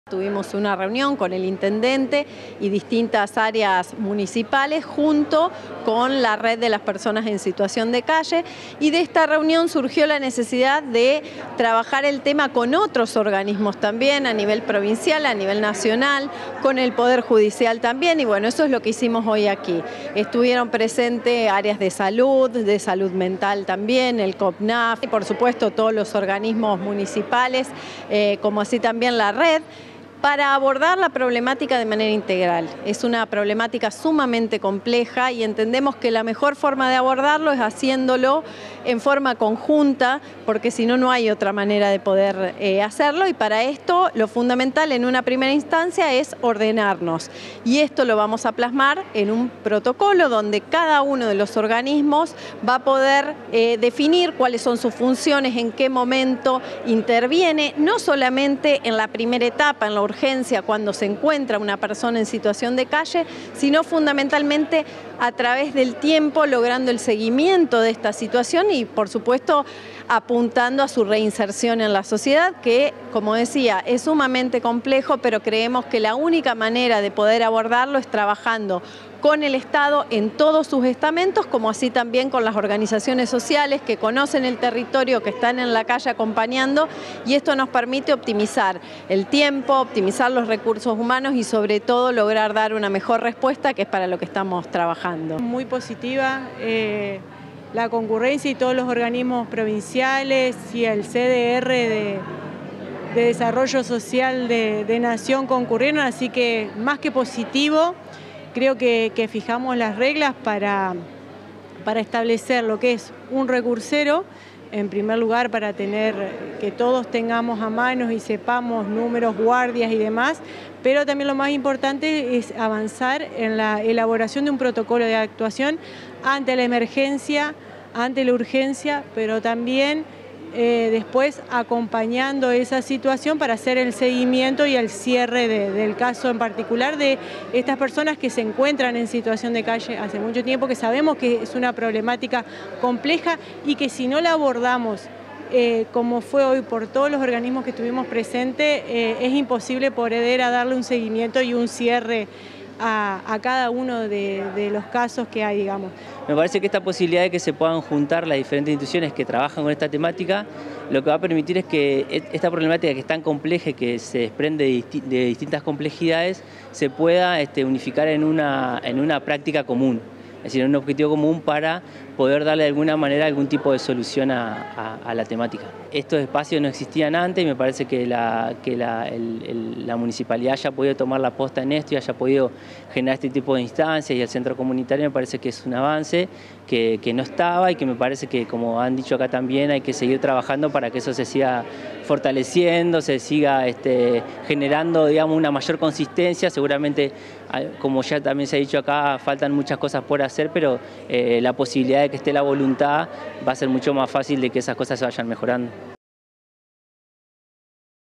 Trabajan en un protocolo de acción para personas en situación de calle
Es una labor conjunta de la Municipalidad y distintos sectores, luego de una convocatoria reciente del Municipio para tratar la temática. También se elaborará un recursero con información de todos los organismos nacionales, provinciales, municipales y sociales con el objetivo de brindar una mejor respuesta en cada caso.